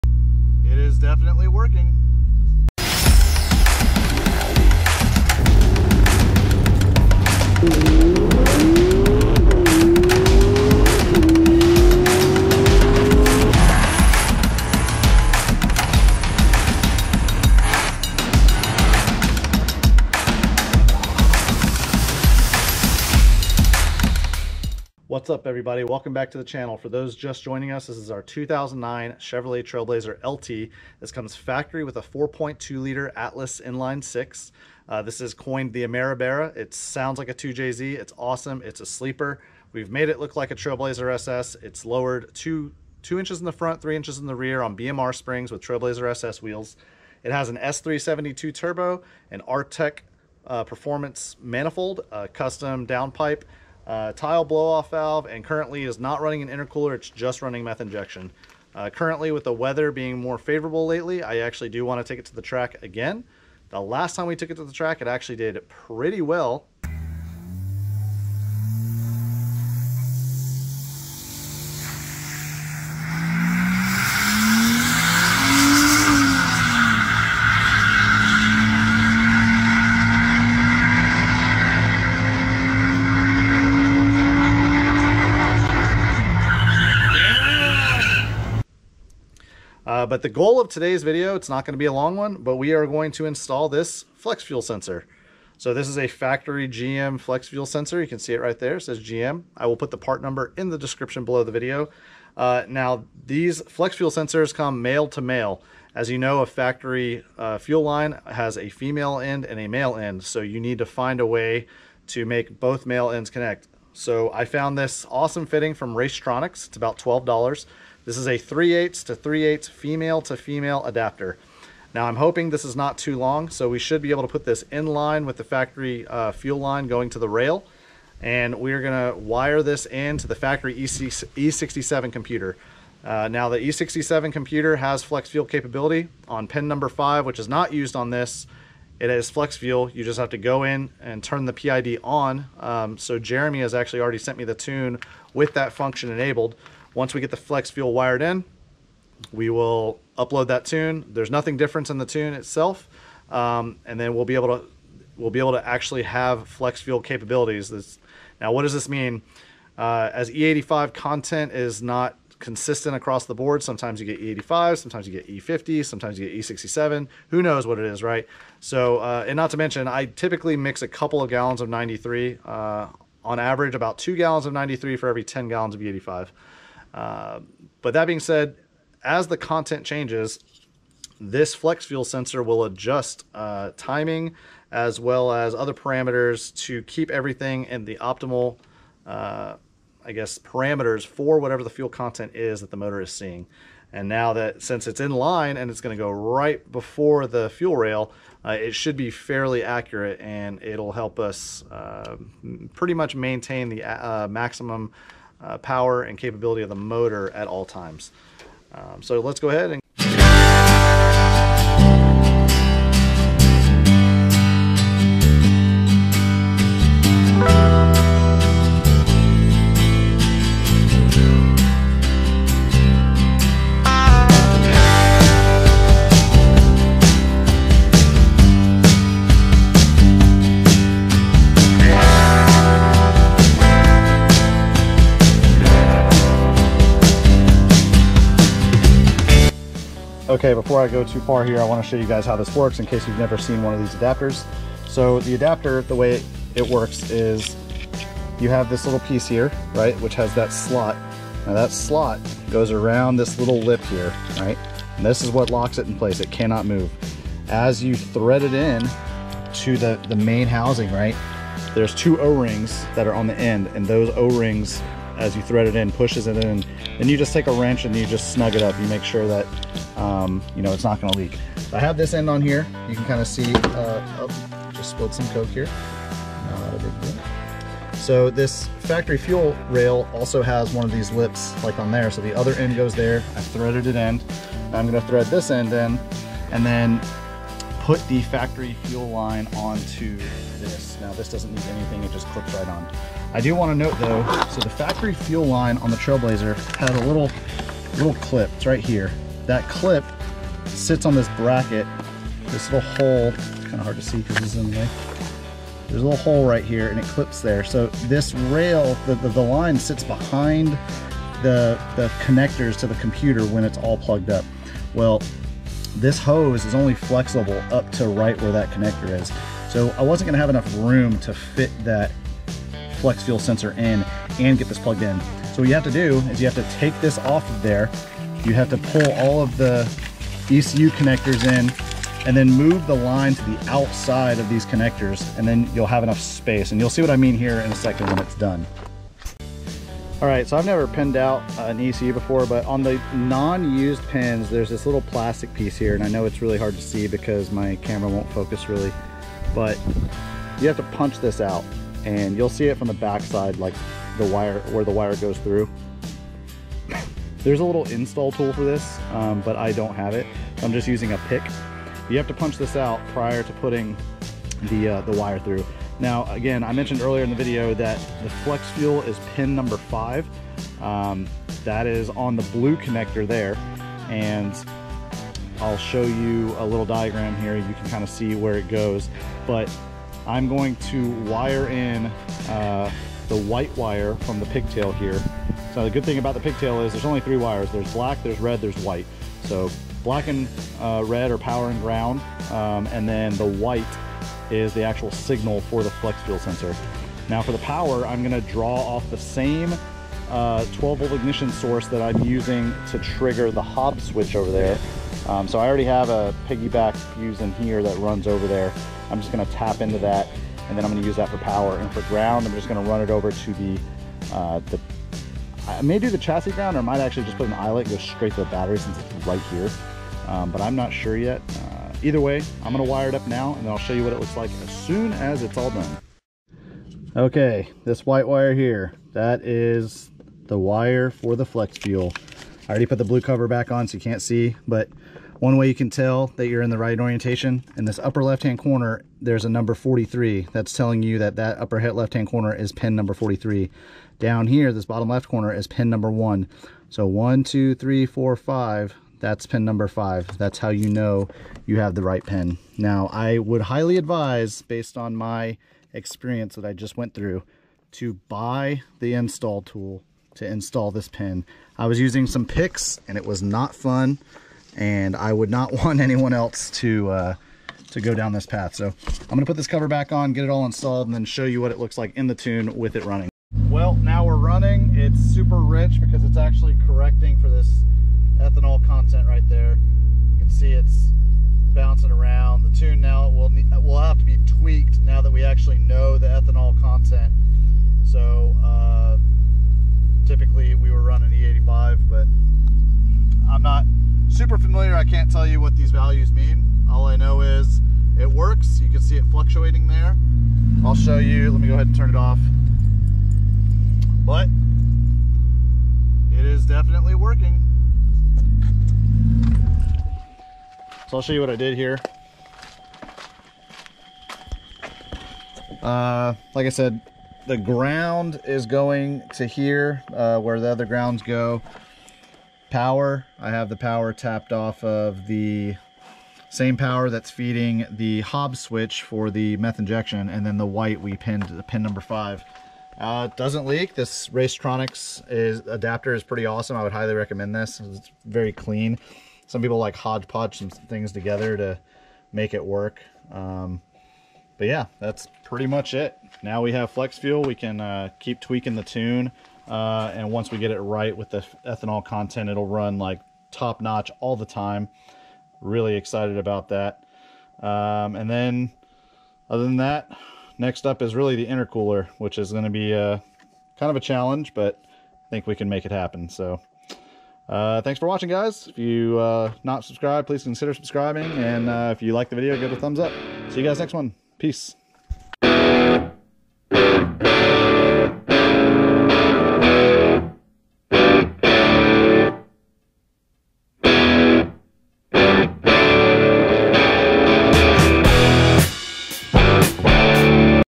Turbo Trailblazer Atlas 4200: Flex Fuel Sensor Install!
This is imperative to making sure the engine is safe in the event the ethanol content is outside of what the vehicle was tuned for. it helps everything work as efficient as possible all the time!

Flex Fuel Sensor

Facebook.com/jwhit54